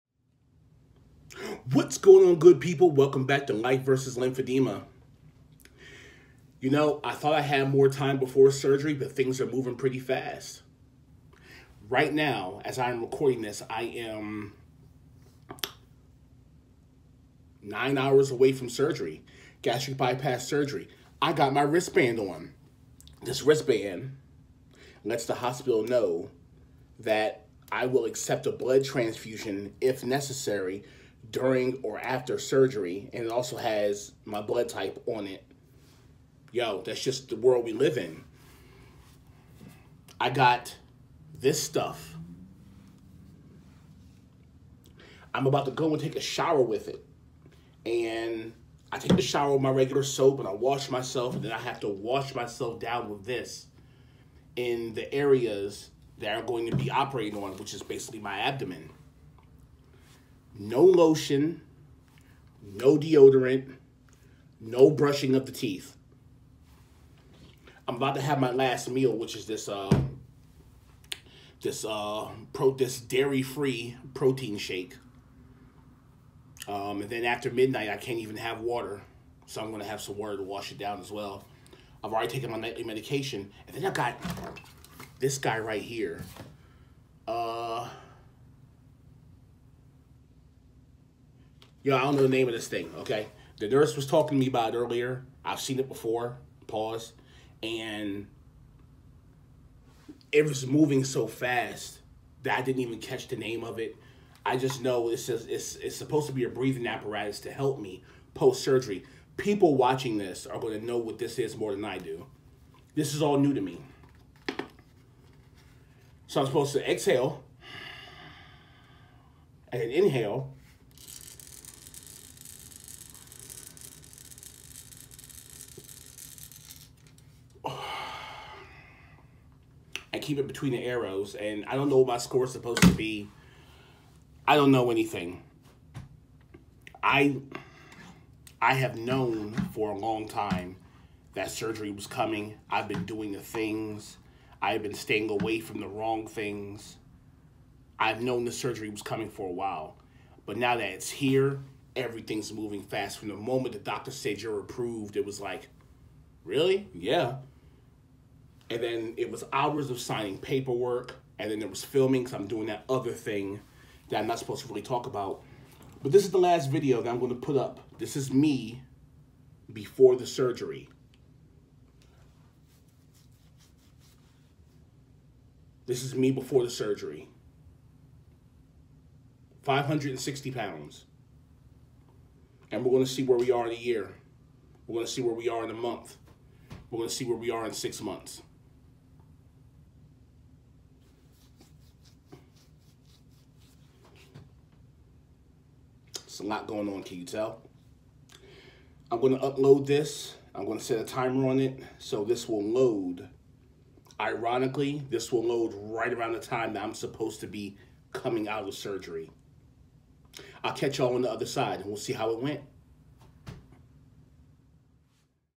Gastric Bypass Final Thoughts
This is the last night before my Gastric Bypass surgery. My final (Sporadic) thoughts, final meal, and final prep. I’ll get back with y’all as soon as I wake up✌🏾🤞🏾